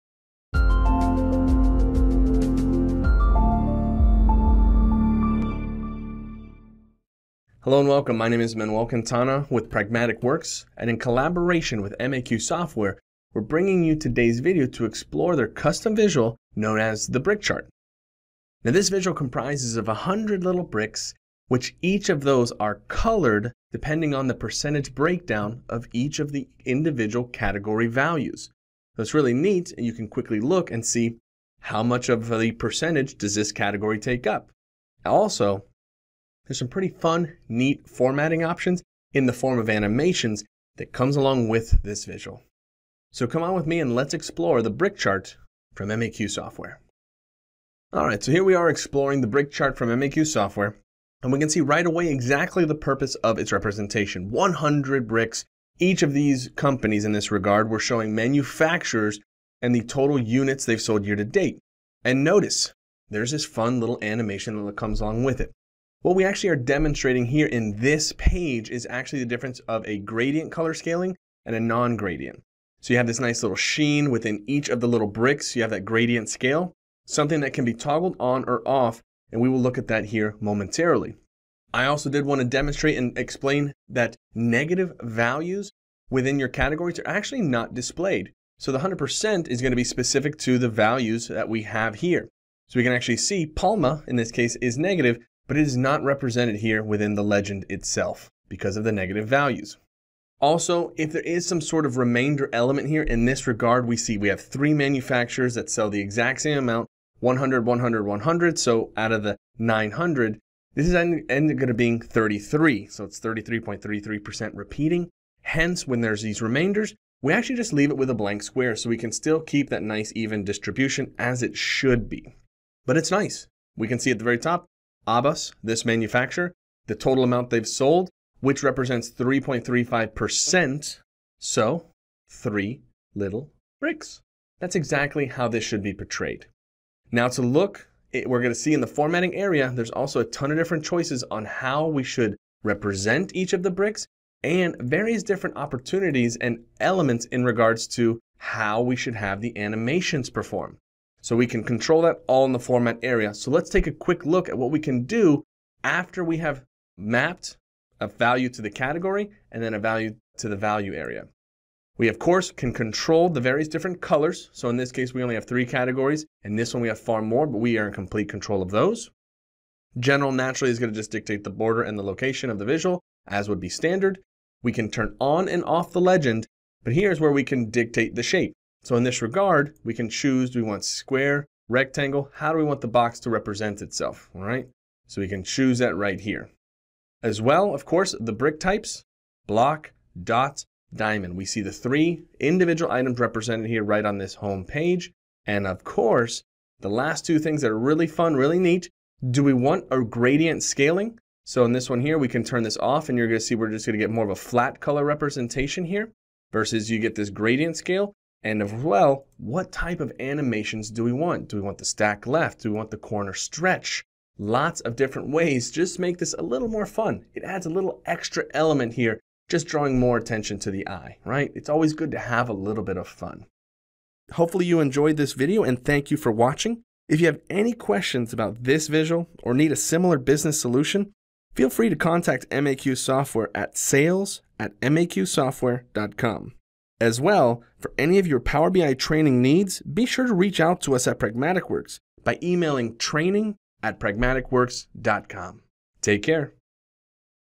Brick Chart by MAQ Software - Power BI Visual Introduction
Brick Chart by MAQ Software allows users to display a percentage breakdown of a category in an alternative format to a donut or pie chart. This visual is useful for showing percentages or positions occupied, such as seats in a legislature. It is also useful for comparing each portion of a category to the whole.

Key features:

Tooltip to indicate the category each color represents and its corresponding percentage.

Option to include a legend with a customizable title, size, and color.

Customizable chart width and height.

** Update **

Brick Chart now supports the Report tooltip and a context menu.